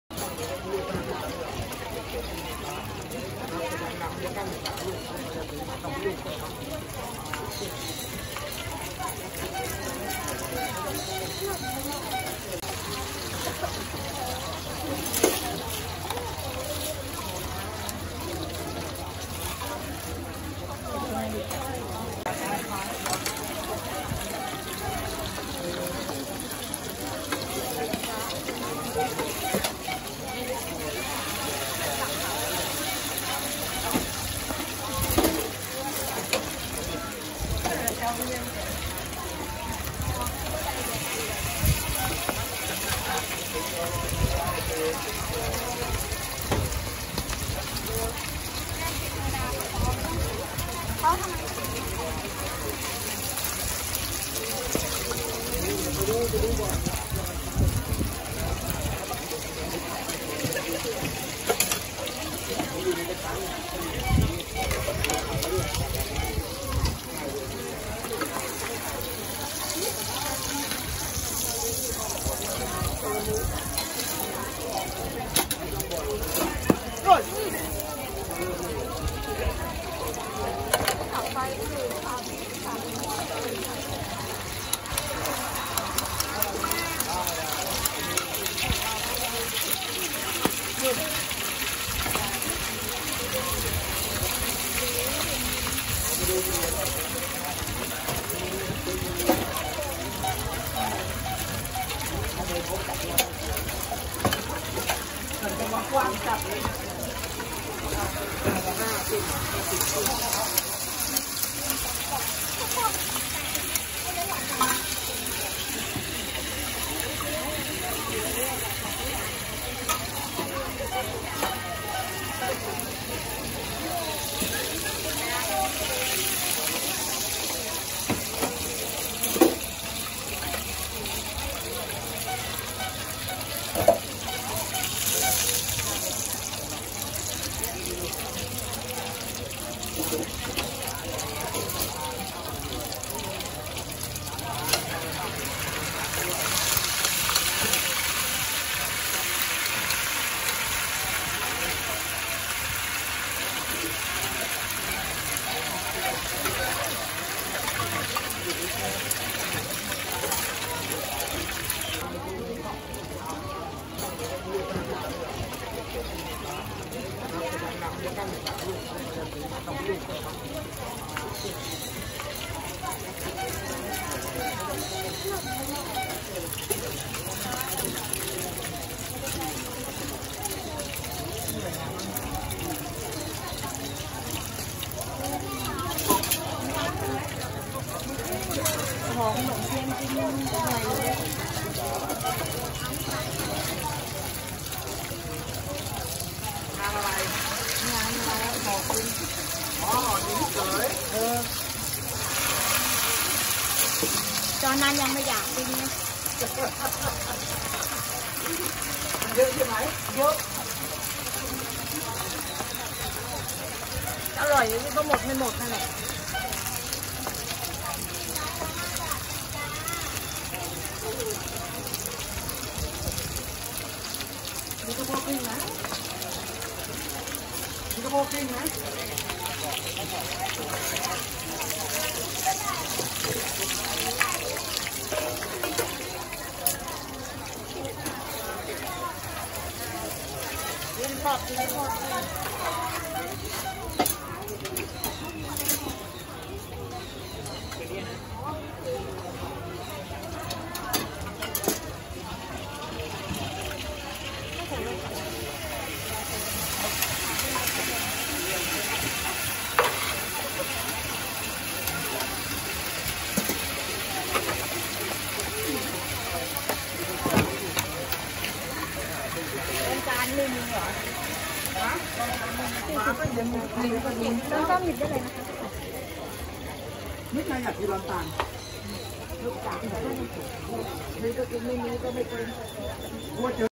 How to Cook Crisp Fried Mussel Pancakes - Crispy Mussel Omelet | Thai Street Food
How to Cook Thai Crisp Fried Mussel Pancakes or Crispy Mussel Omelet aka "Hoi Tod"
Price: 40 Baht (1.3 USD)